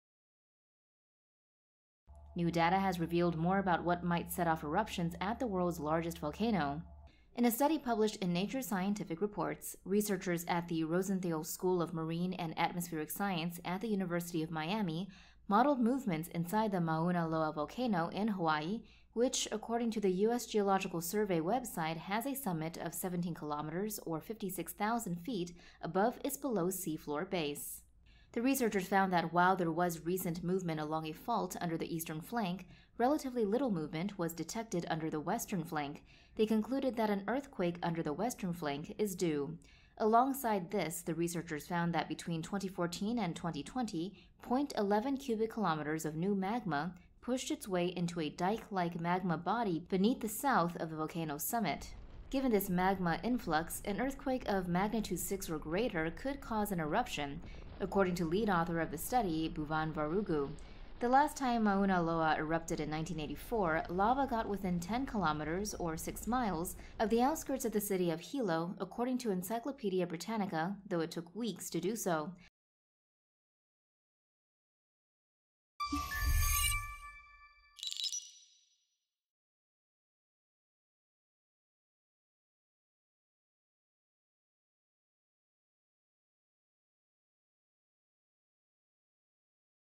What would it take for the world's largest volcano to erupt?
New data has revealed more about what might set off eruptions at the world's largest volcano.

In a study published in Nature Scientific Reports, researchers at the Rosenstiel School of Marine and Atmospheric Science at the University of Miami modeled movements inside the Mauna Loa volcano in Hawaii, which according to the U.S. Geological Survey website has a summit 17 kilometers, or 56,000 feet, above its below-sea-floor base.

The researchers found that while there was recent movement along a fault under the eastern flank, relatively little movement was detected under the western flank. They concluded that an earthquake under the western flank is due.

Alongside this, the researchers found that between 2014 and 2020, 0.11 cubic kilometers of new magma pushed its way into a dike-like magma body beneath the south of the volcano's summit.

Given this magma influx, an earthquake of magnitude 6 or greater could cause an eruption, according to lead author of the study Bhuvan Varugu. The last time Mauna Loa erupted in 1984, lava got within 10 kilometers, or 6 miles, of the outskirts of the city of Hilo, according to Encyclopedia Britannica, though it took weeks to do so.

RUNDOWN SHOWS:
1. Location of Mauna Loa volcano
2. Cross section of volcano
3. Movement of volcano's eastern and western flanks
4. Creation of new magma chamber
5. Eruption and lava flow in 1984

SOURCES: USGS, Rosenstiel School of Marine and Atmospheric Science, Nature.com, Encyclopedia Britannica